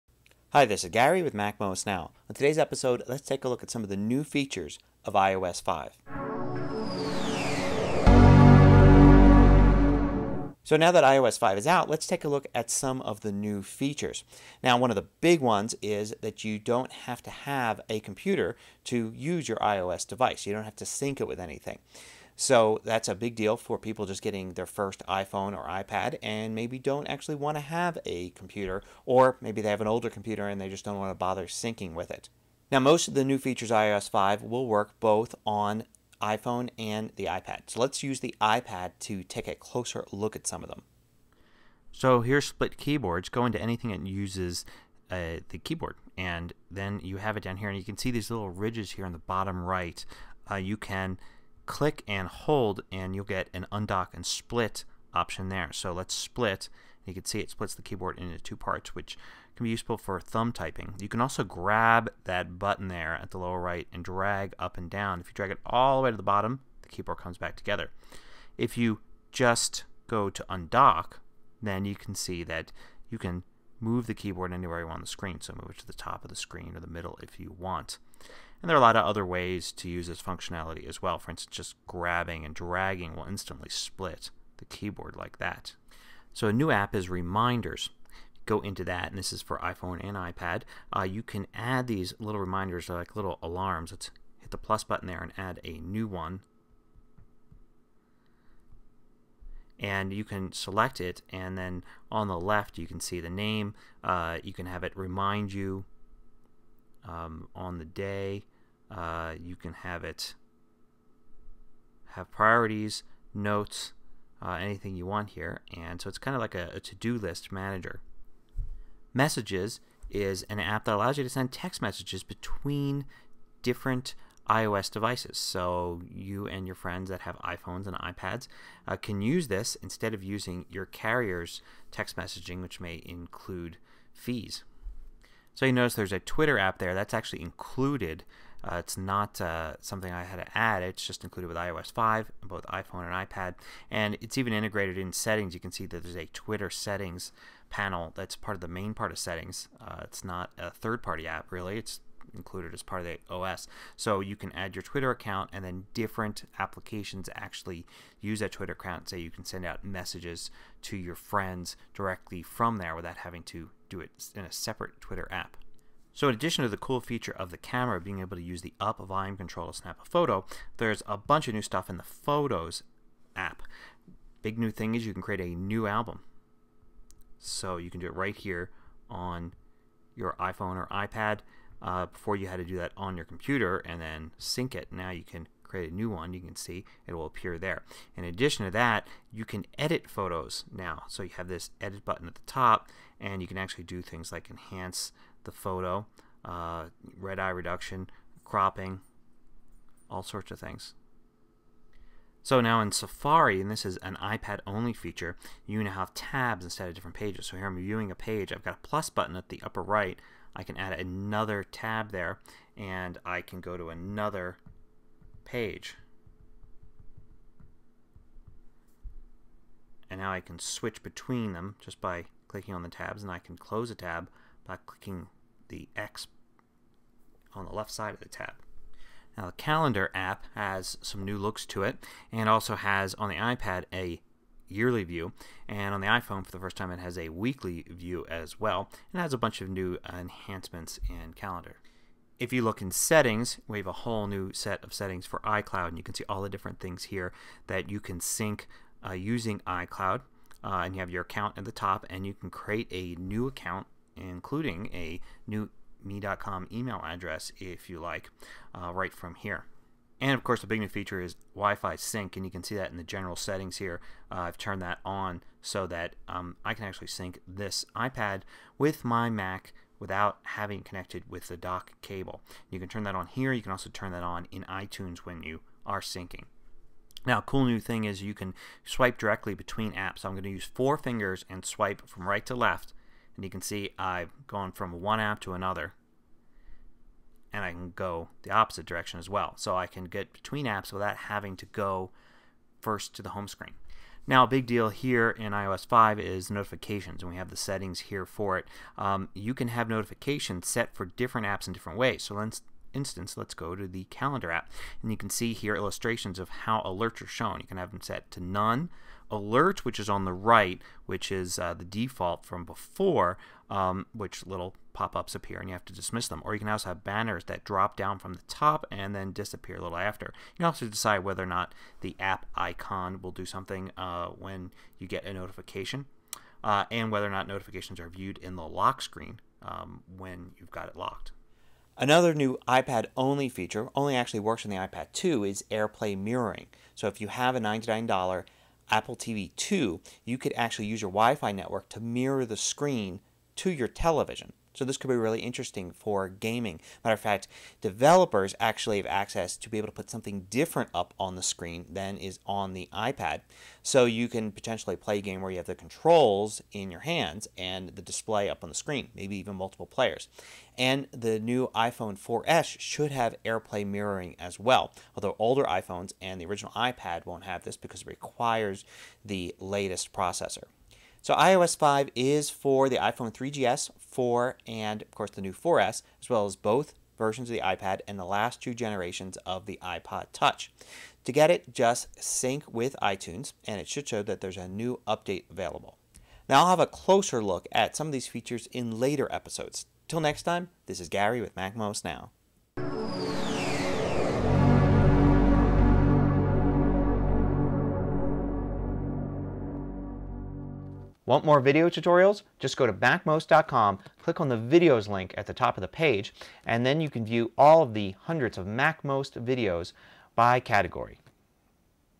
iOS 5 First Look (MacMost Now 617)
Take a look at the new features in iOS 5. New apps like Reminders and Messages add new functionality. Notifications now have a wider set of options. Twitter is built in to many apps. You can move and split the on-screen keyboard. You can sync with iCloud and use an iOS device independent of any computer.